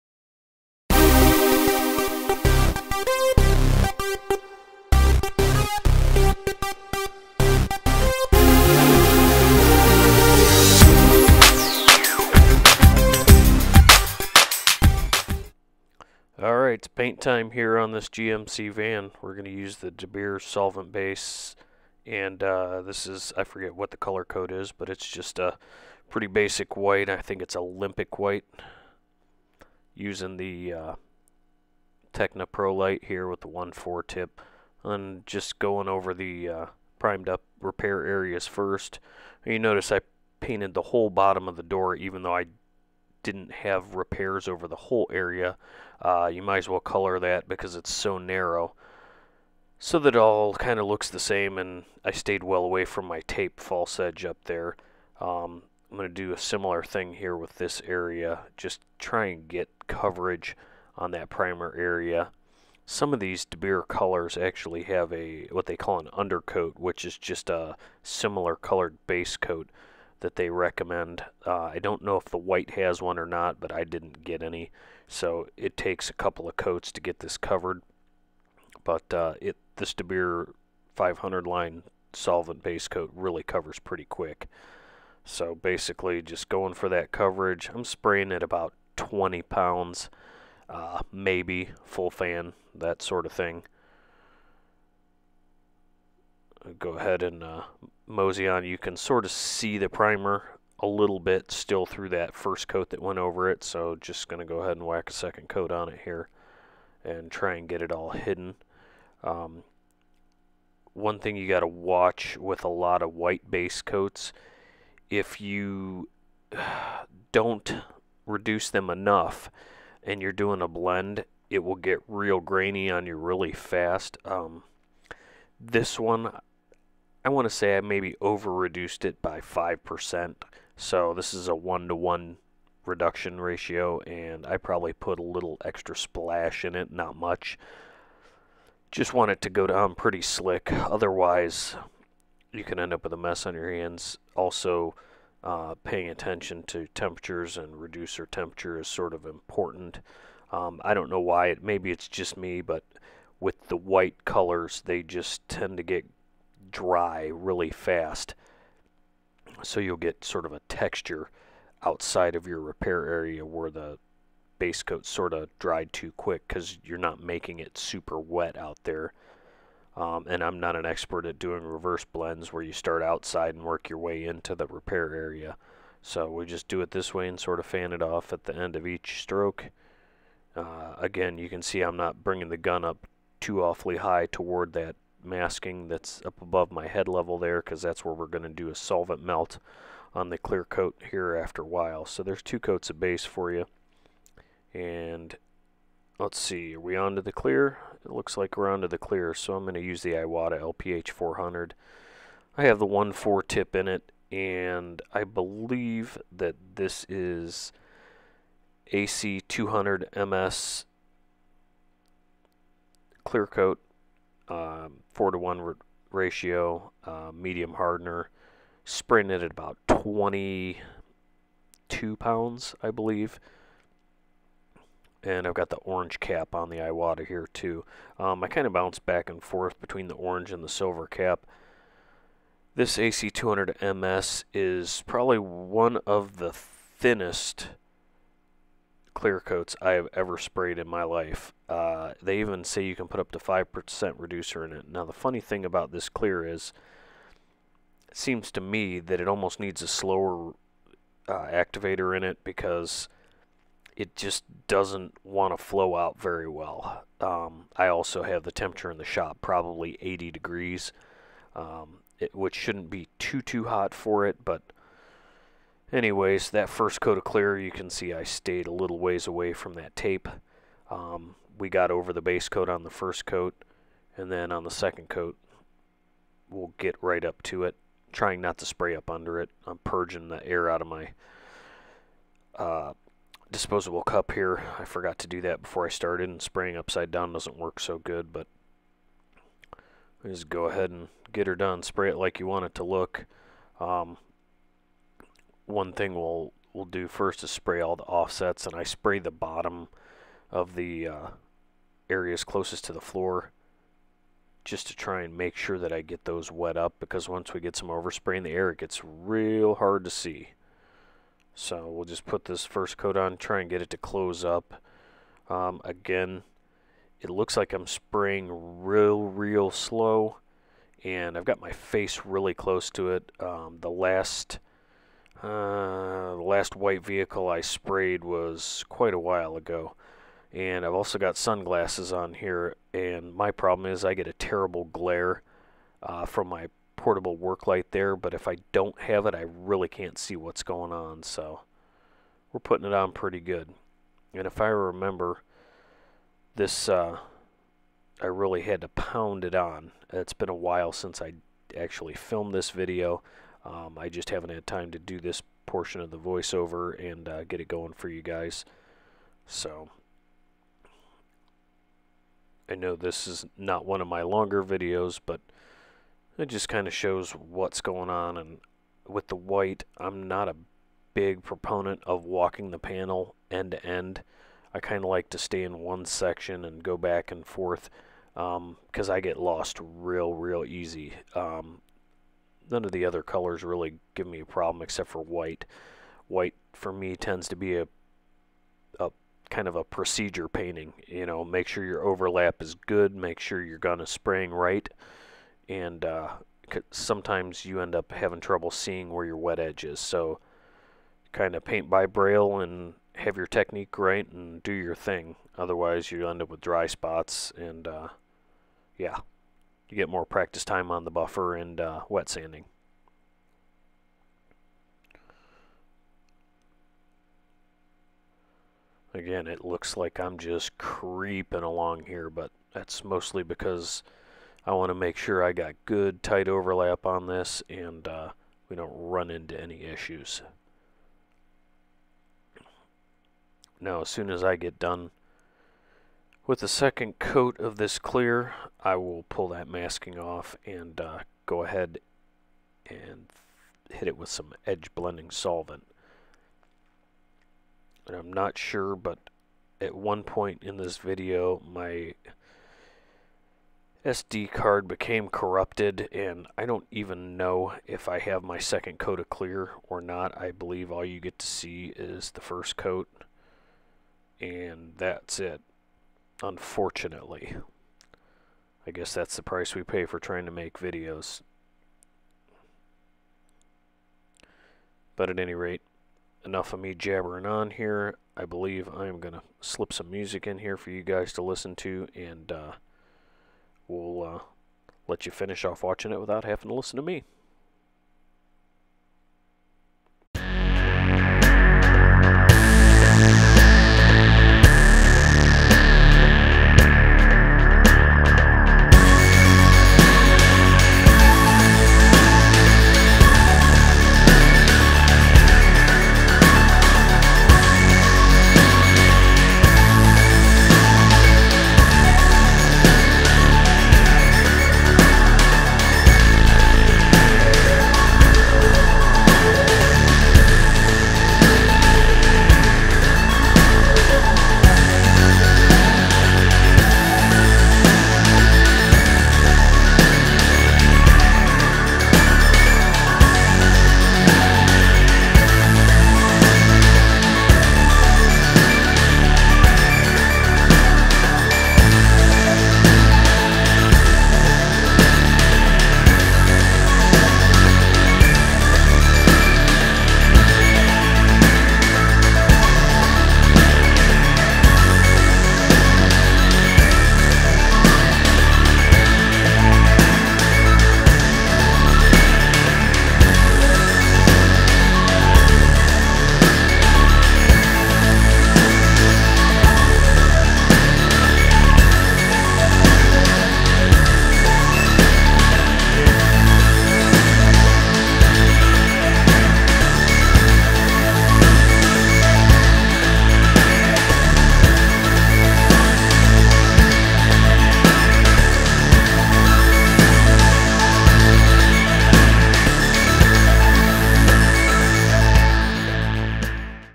Basecoat and Clearcoat on the GMC van
Getting some paint on the GMC Savana Van.  I really hate painting white but this one turned out ok after a cut and buff.  Sorry there is no finished video of this one due to it having to leave the shop sooner than expected.
Base Coat - DeBeer 500 solvent basecoat sprayed with Tekna pro-lite Te20 aircap at 20 psi 1.4 tip
Clearcoat - Valspar AC 200 MS 4:1 sprayed with Iwata LPH 400 LV4 aircap and 1.3 tip at 22 psi